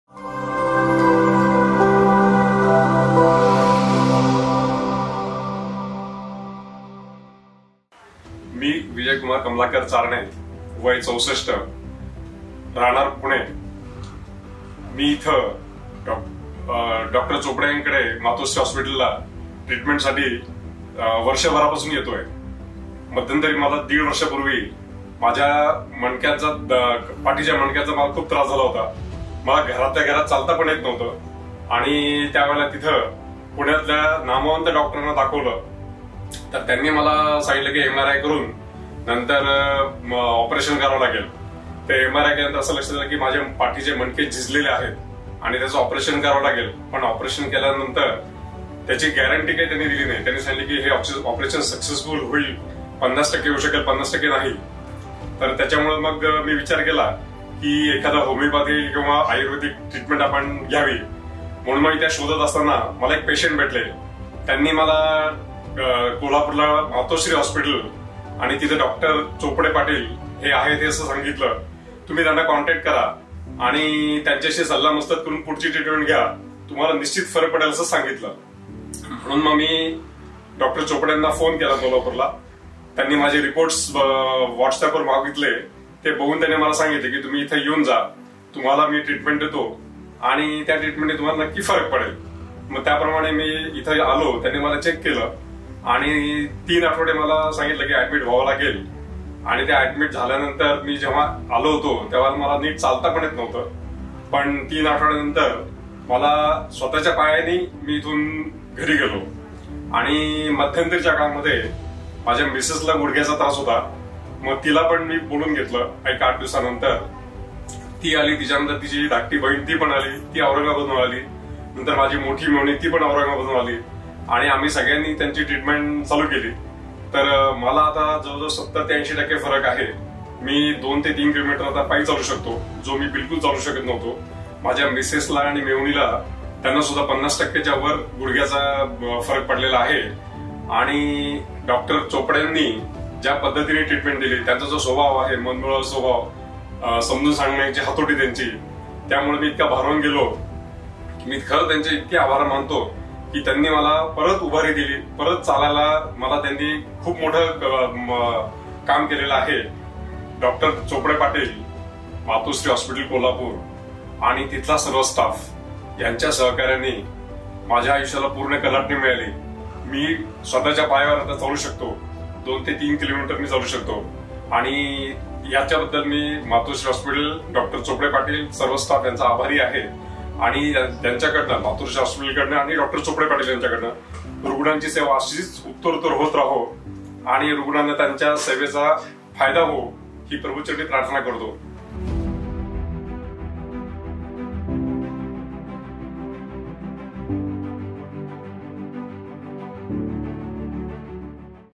Lumbar Spondylosis, Knee Degeneration & Rheumatoid Arthritis Patients Cured By Matoshree Homoeopathy
Homeopathy Dr. Dattatray Angad Chopade Patil MD Hom.
Dr. Pooja Dattatray Chopade Patil MD Hom.

Mr Vijaykumar Charane and family Pune & Aurangabad
Lumbar Spondylosis, Knee Degeneration & Rheumatoid Arthritis Homoeopathic Treatment